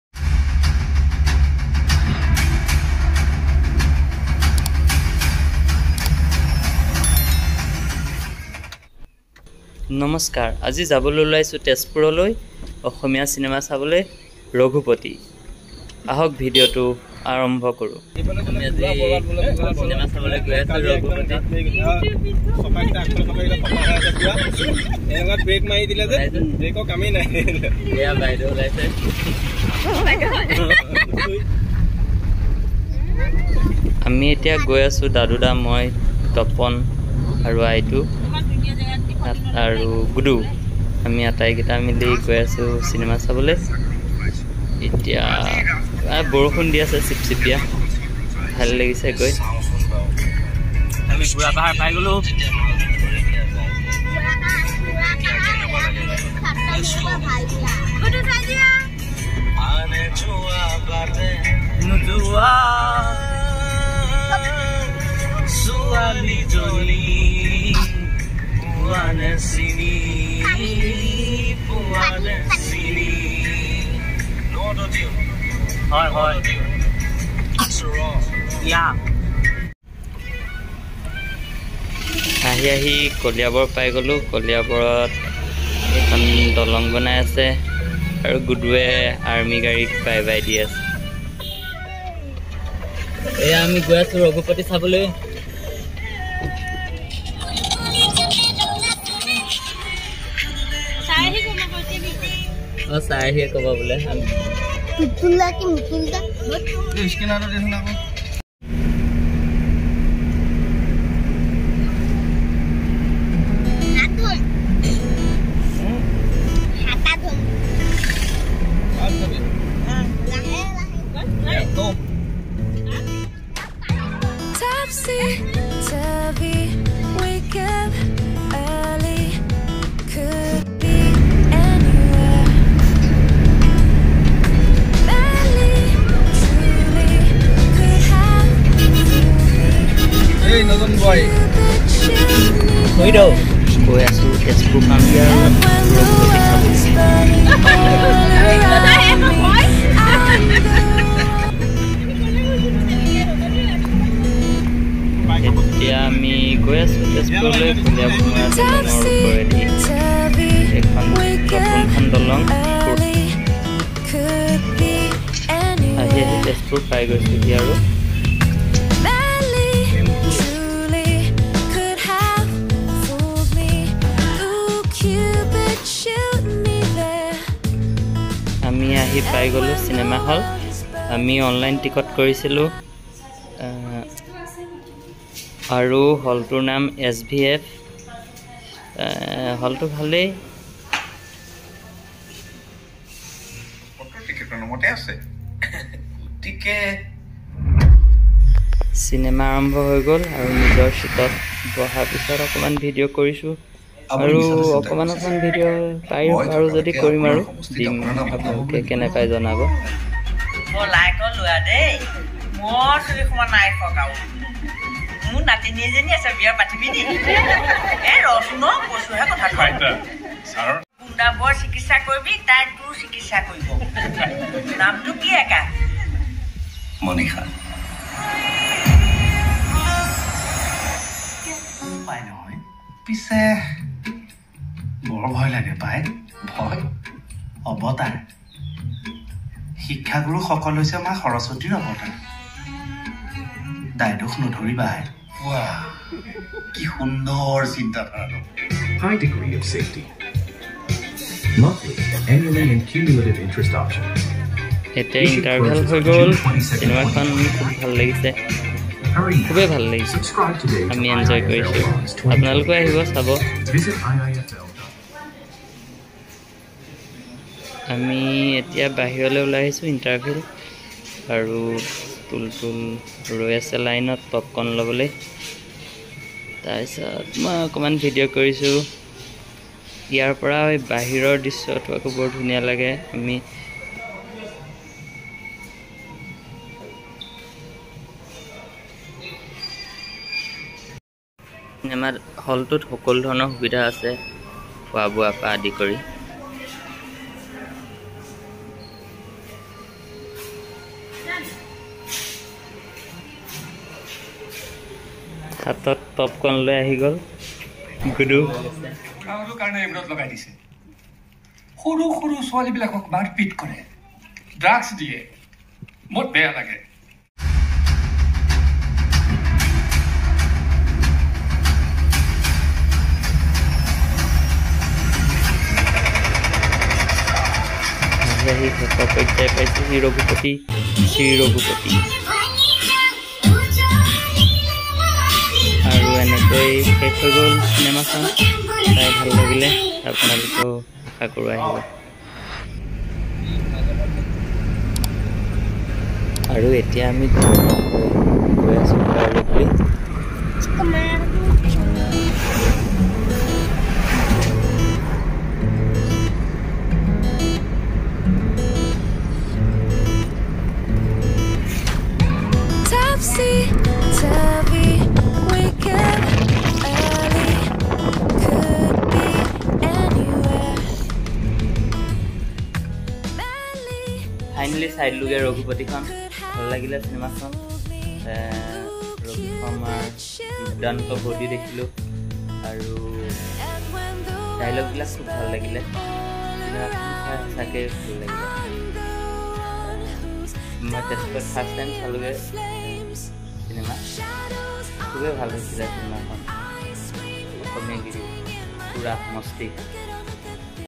Raghupati Assamese Movie // শ্ৰী ৰঘুপতি অসমীয়া চিনেমা চাবলৈ গ'লো তেজপুৰ SVF Cinama.
Raghupati
Shri Raghupati
Assamese Cinama
Assamese movies